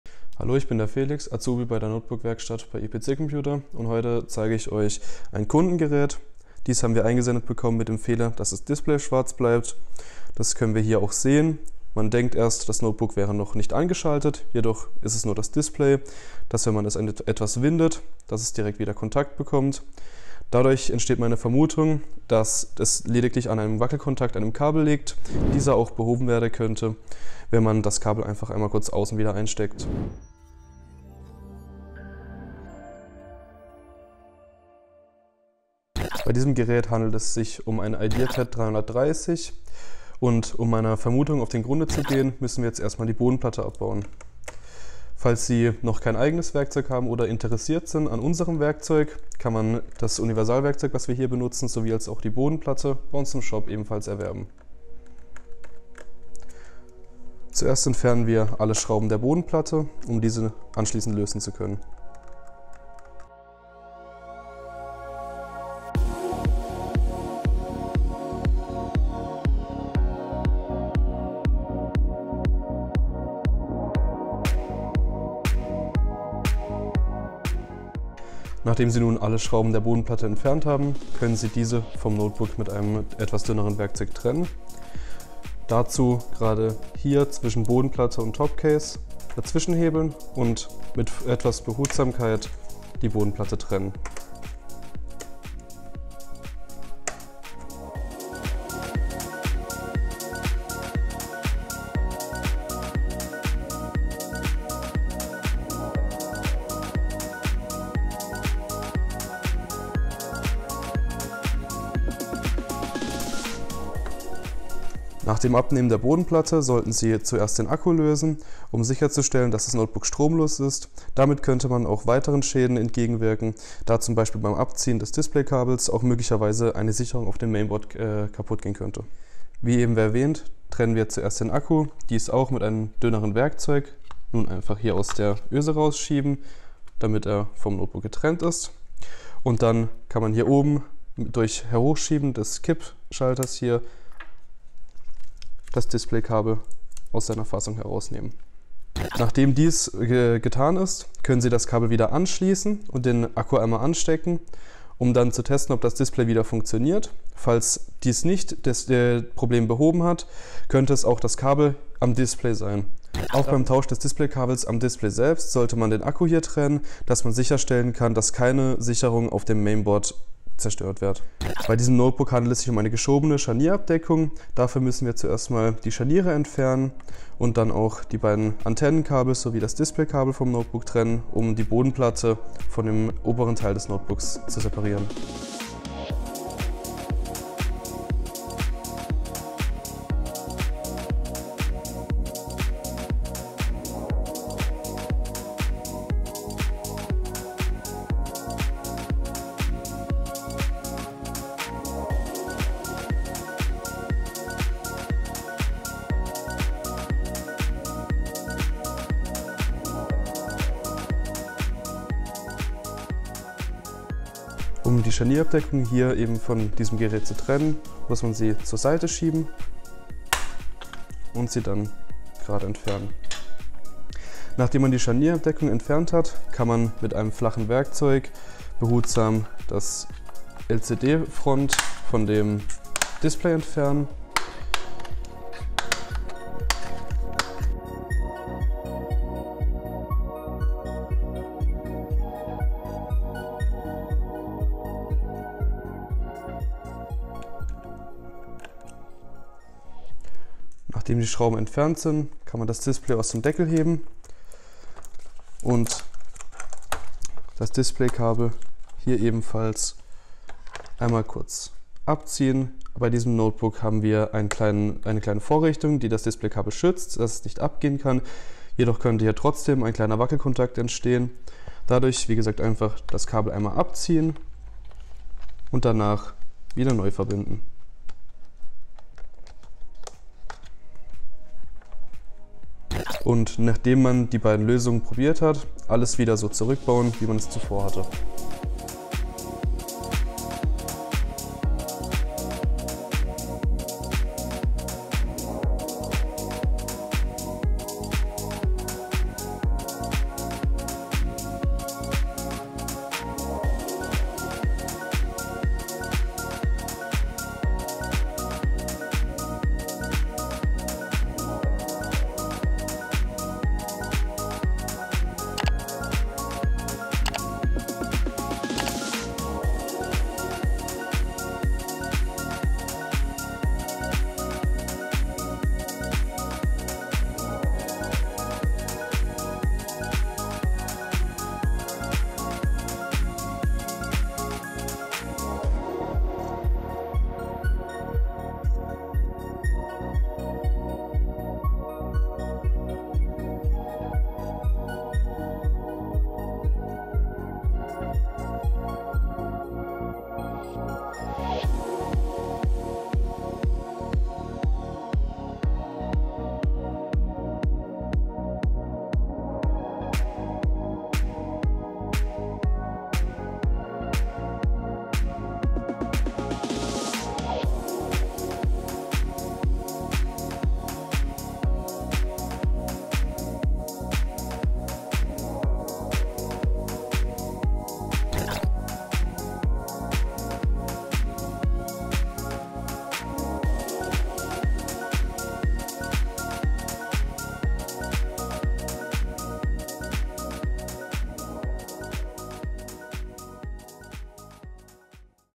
Lenovo ThinkPad - Bildschirm geht nicht an durch Wackelkontakt am Displaykabel
Eine häufige Problemdiagnose unserer Kunden gehen in diese Richtung - "Bildschirm bleibt schwarz", "Display flackert", "Laptop geht nur in einem bestimmten Winkel"
In diesem Video erklären wir einen geläufigen Lösungsansatz für Probleme dieser Art. Unser Azubi Felix zeigt euch, wie ihr beide Seiten des Displaykabels auf möglichen Wackelkontakt oder Kabelbruch checken könnt.

🔧Gibt es doch Probleme könnt ihr euer Gerät auch für eine professionelle Reparatur anmelden: